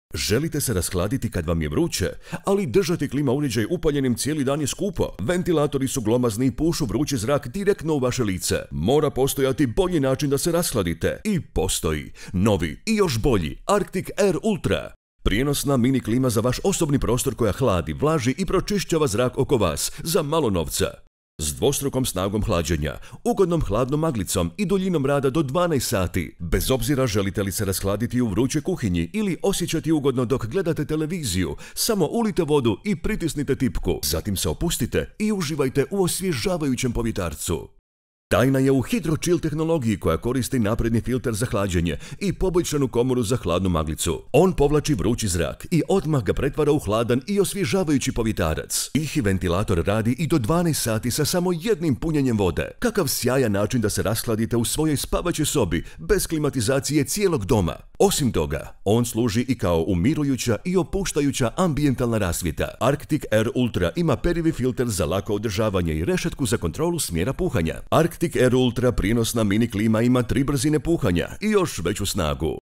Prijenosna mini klima Arctic Air Ultra
Zahvaljujući naprednoj tehnologiji, tihom radu i 3 različite jačine hlađenja možete osvježiti zrak gdje god se nalazili.